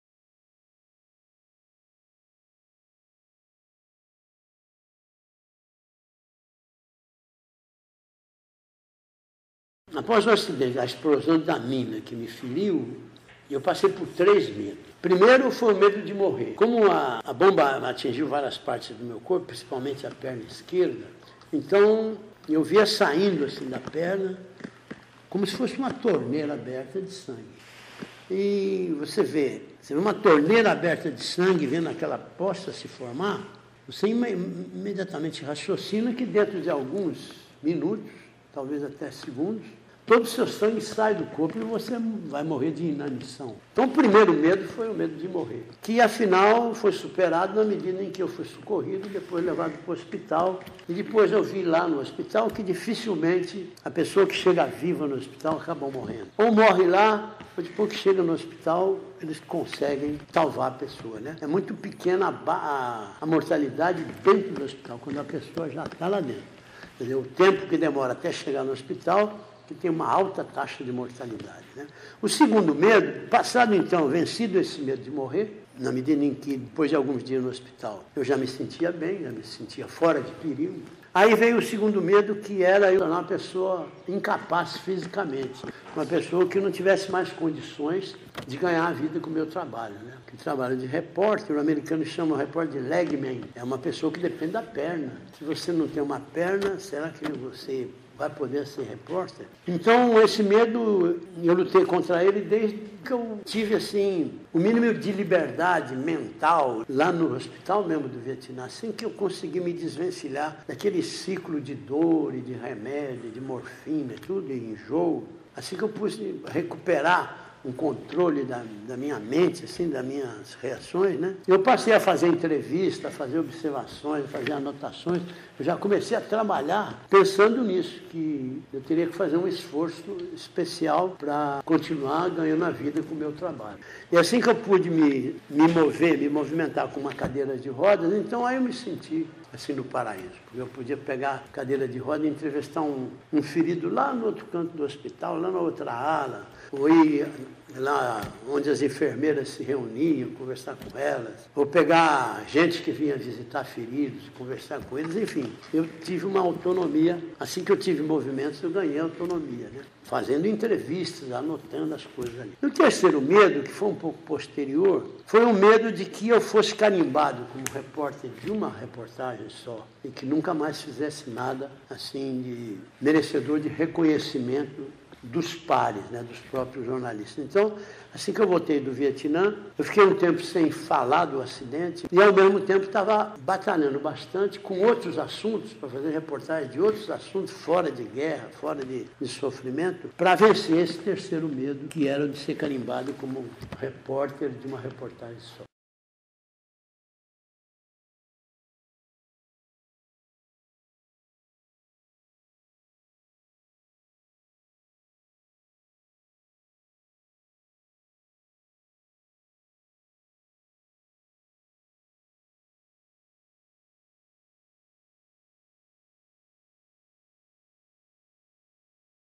José Hamilton Ribeiro
O consagrado jornalista José Hamilton Ribeiro concedeu entrevista exclusiva a equipe 'Um mundo de notícias' e revelou quais foram os seus grandes medos quando foi atingido por uma mina terrestre no Vietnã.
Um Mundo de Notícias  faz parte do projeto de conclusão de curso  sobre Jornalismo Internacional das alunas do 8° semestre de jornalismo da Unimep: Carla Bovolini, Daniane Gambaroto, Milena Barros e Thiane Mendieta.